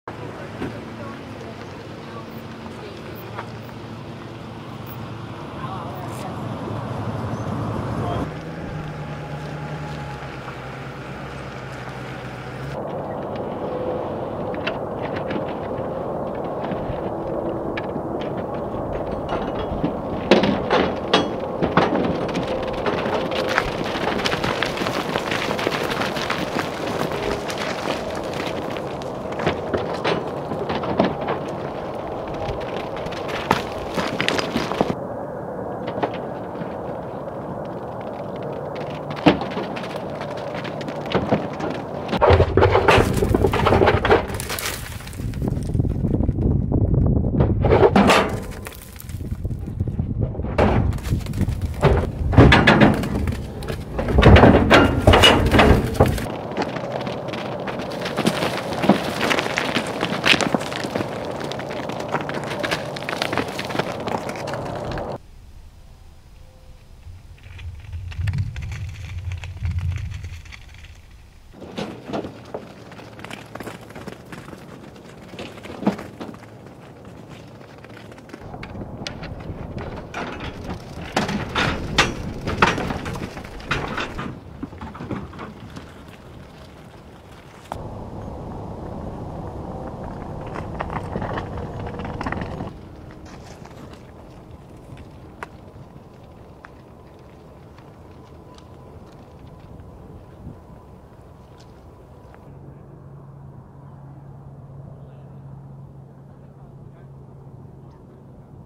Bighorn sheep returned to historic habitat
Dozens of bighorn sheep were relocated as part of an effort to re-establish bighorn populations in their natural habitat.